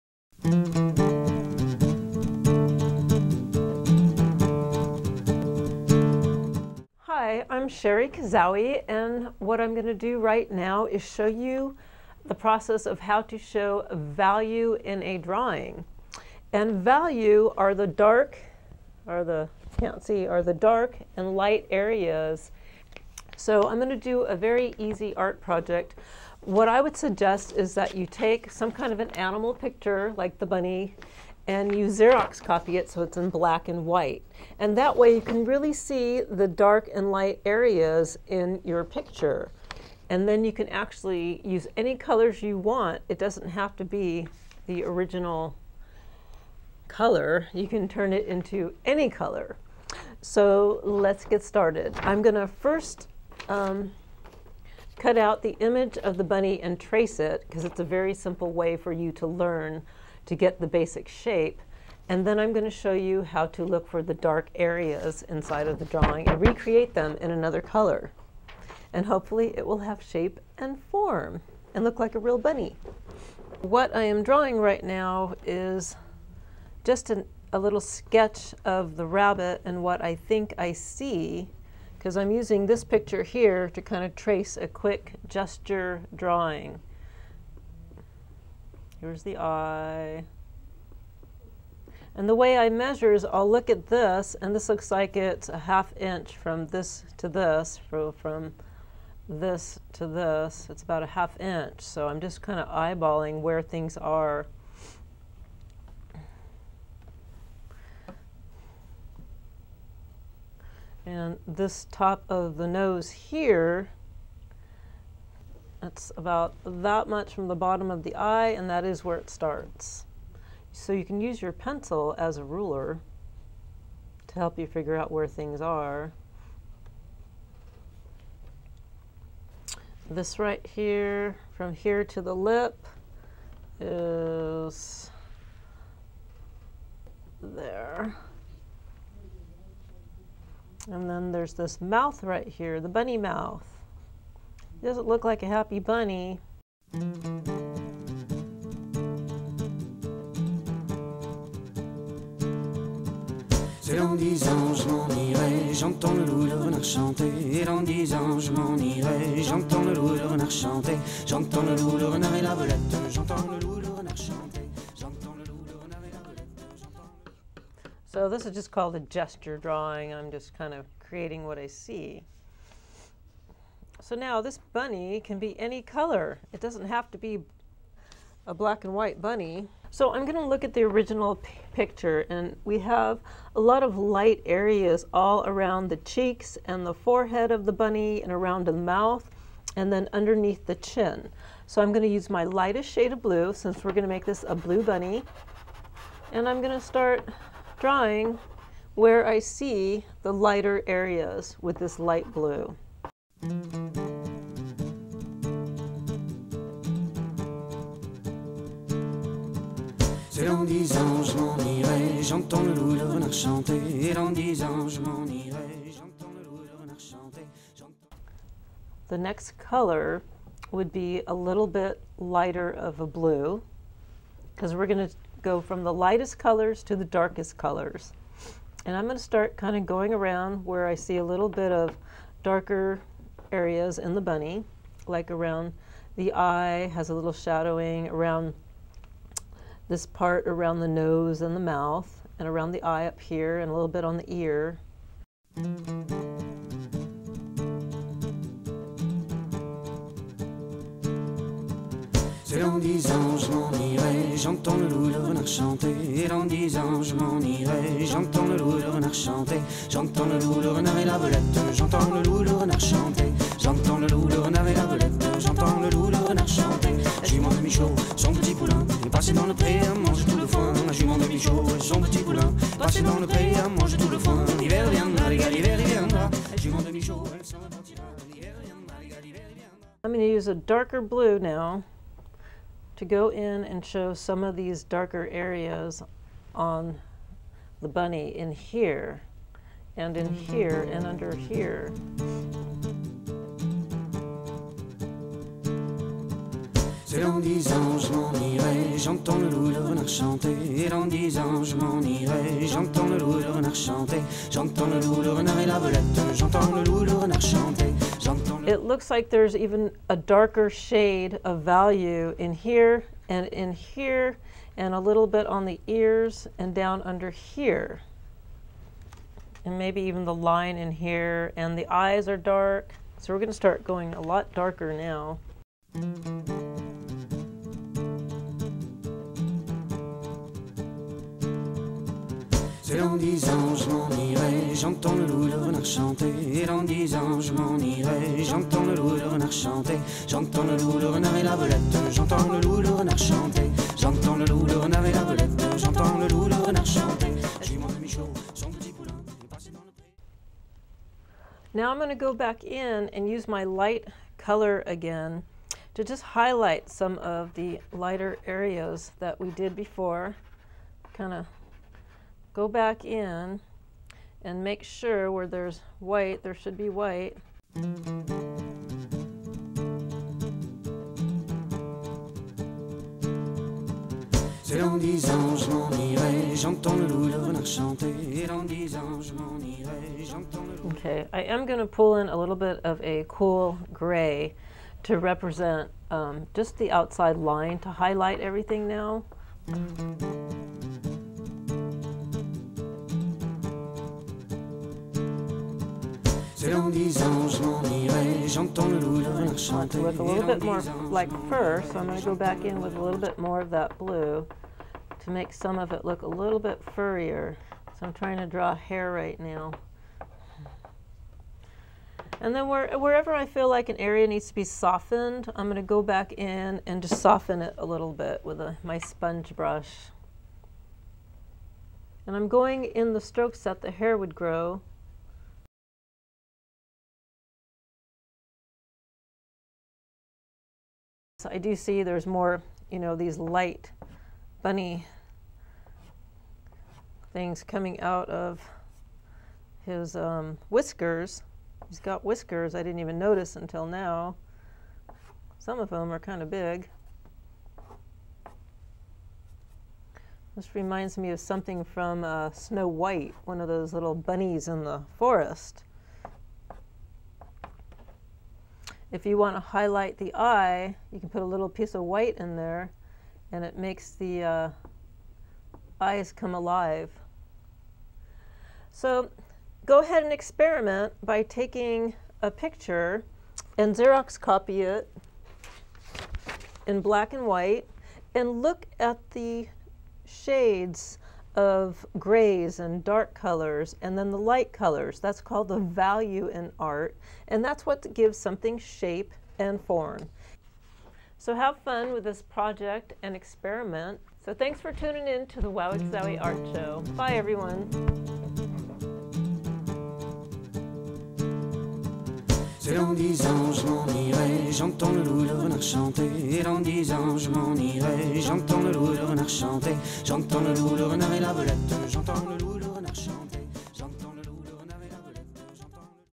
Wow Wee Kazowie, Draw a Blue Bunny (made easy)
Draw a blue bunny, A bunny picture can be any color. Learn to give a drawing shape and form by understanding the VALUE in a drawing.This is an art demonstration showing the light and dark colors in a drawing to give it shape and form, by Shari Kazowie from" The Wow Wee Kazowie Art Show".